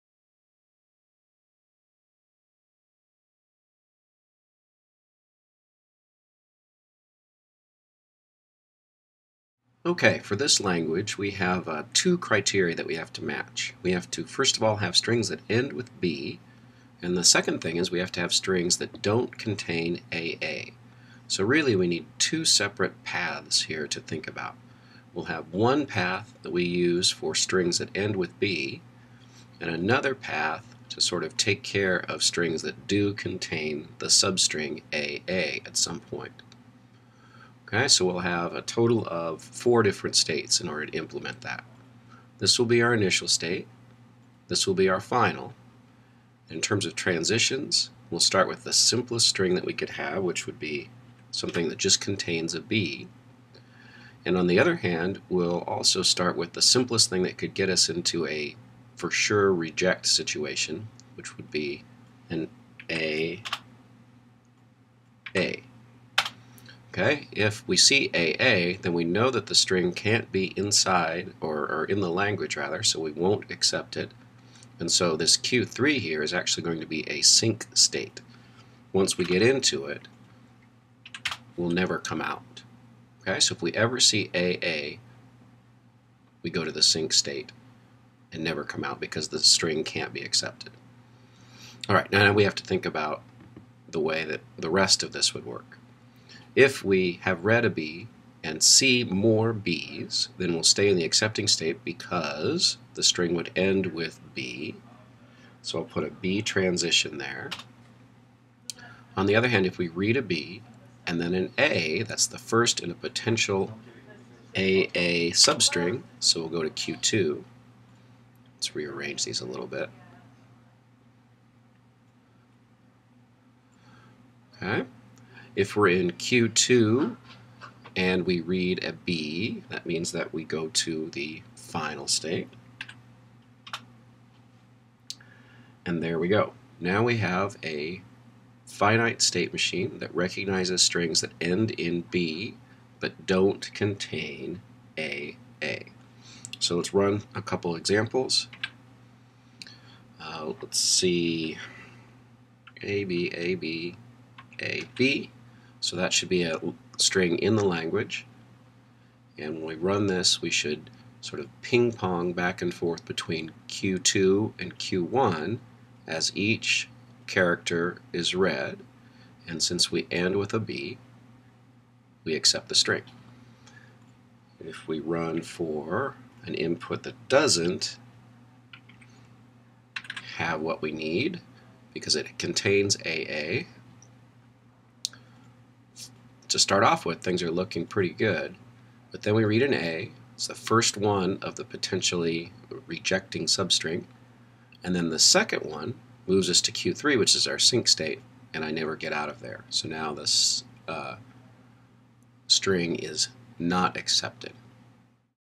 Finite automata to accept strings ending with b, but not containing aa.
This is a deterministic finite automata that accepts the language of strings ending with b, but not containing aa.